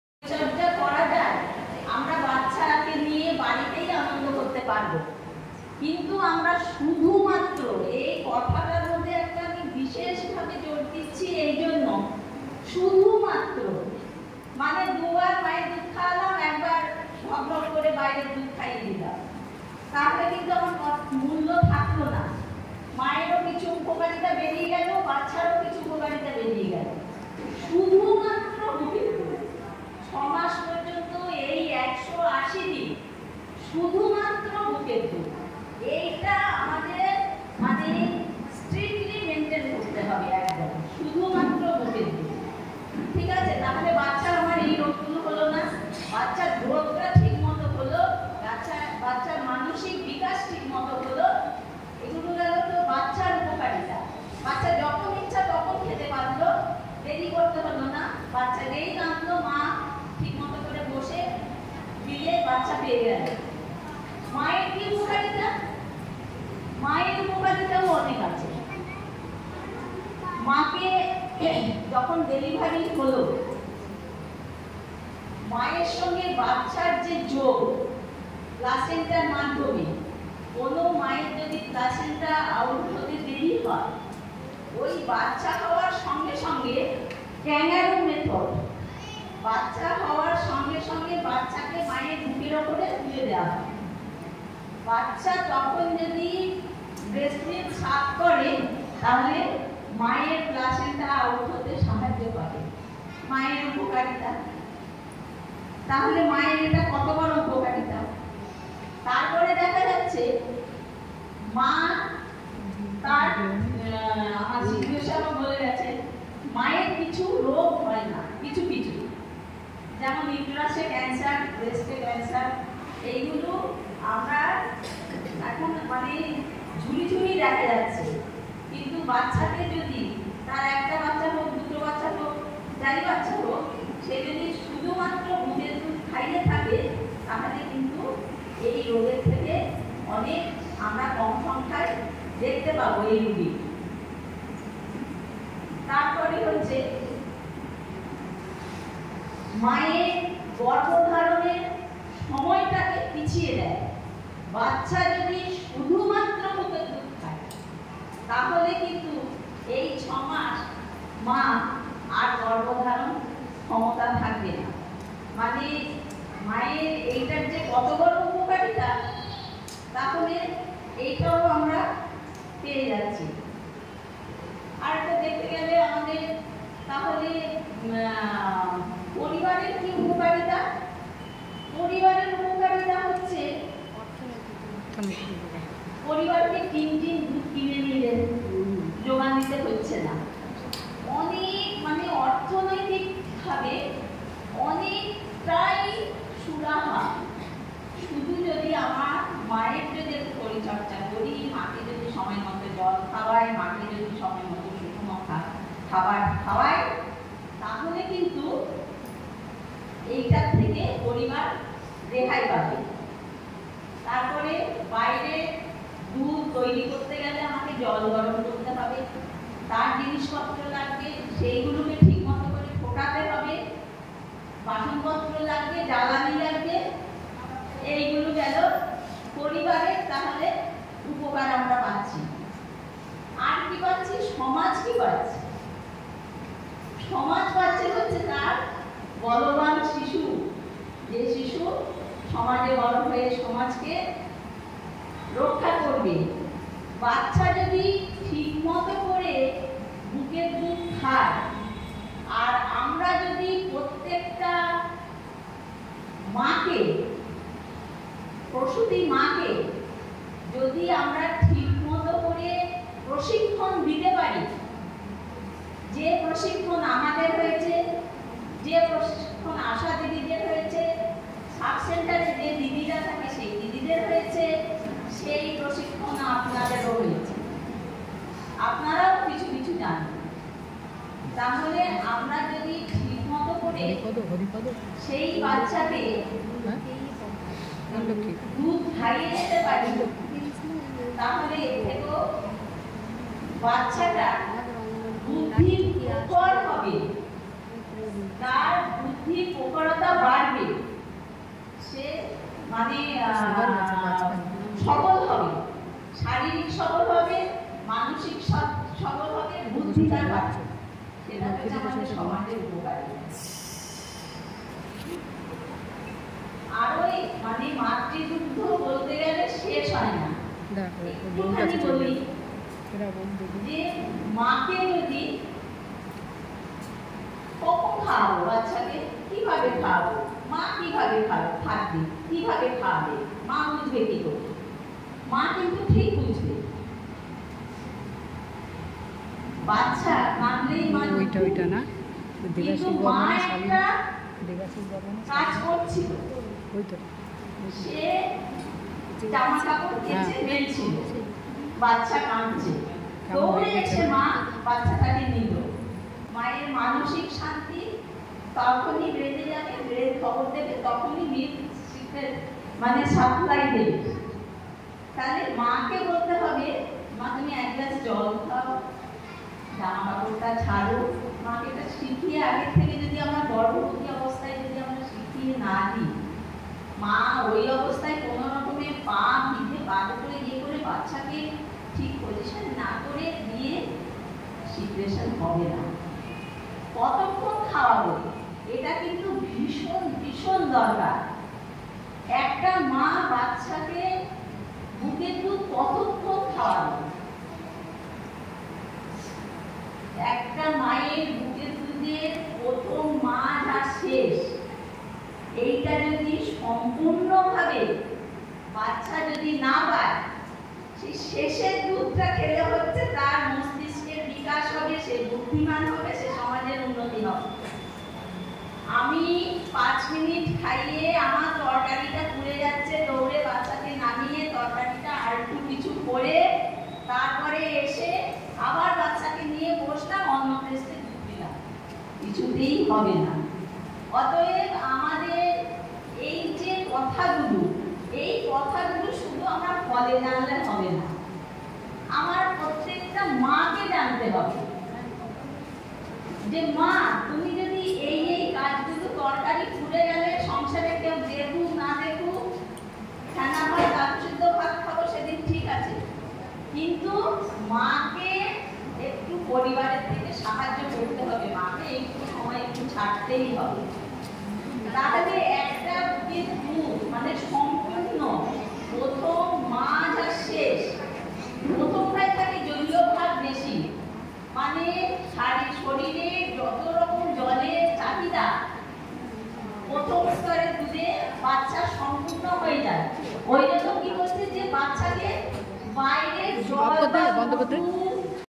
মাতৃদুগ্ধ পানের গুরুত্ব। Importance of breast feeding.Discuss by PHN-Health Department.
Importance of breast feeding.Discuss by PHN-Health

World breast feeding week-2023 celebration